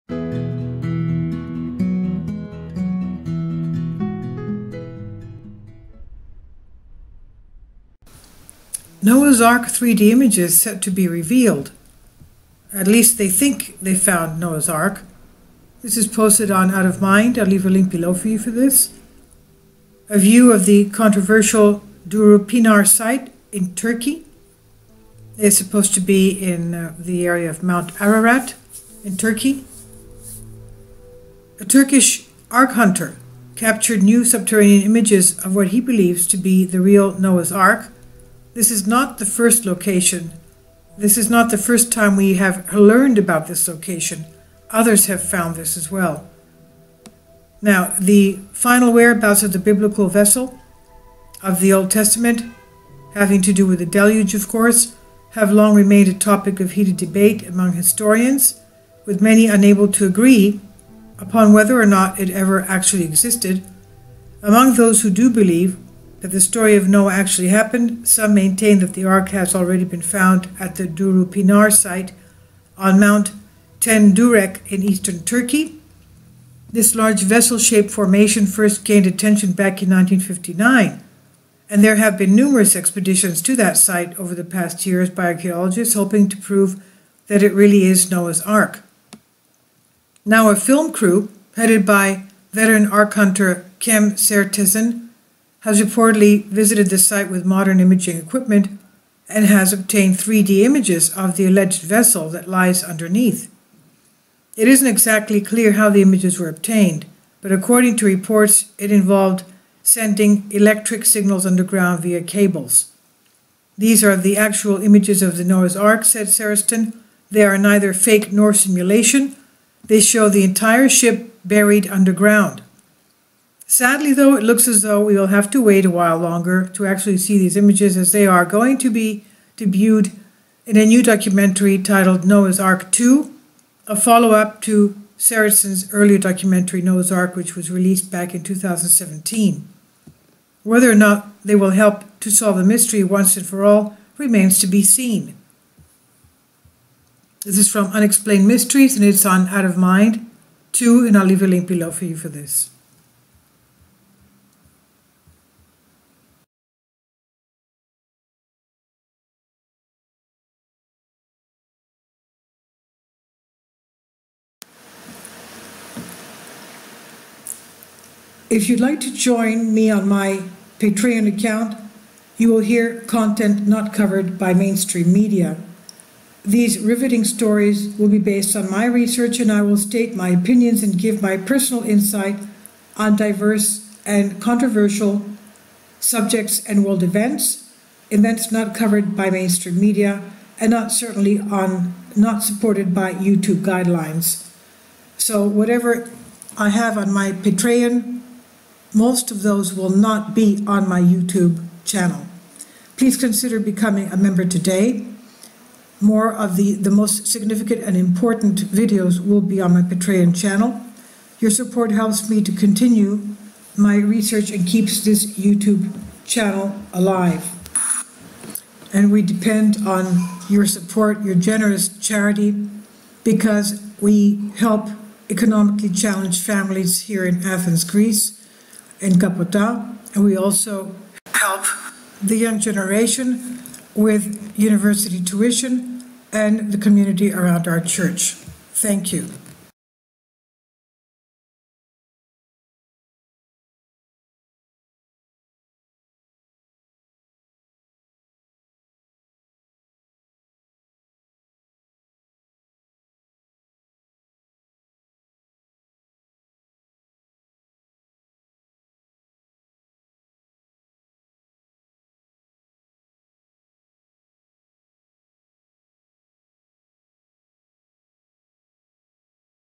'Noah's Ark' 3D Images Set To Be REVEALED in Documentary!
3D images of 'Noah's Ark' set to be revealed